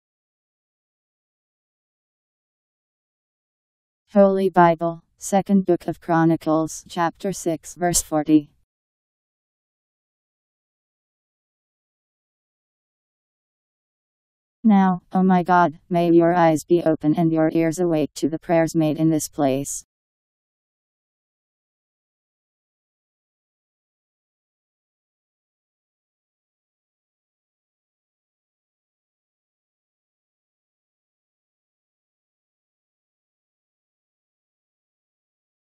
Holy Bible Second Book Of Chronicles 6:40
Video shows Holy Bible Second Book Of Chronicles Chapter 6 Verse 40: Now, O my God, may your eyes be open and your ears awake to the prayers made in this place. Powered by MaryTTS. Text taken from Bible in Basic Edition. Bible Second Book Of Chronicles 6:40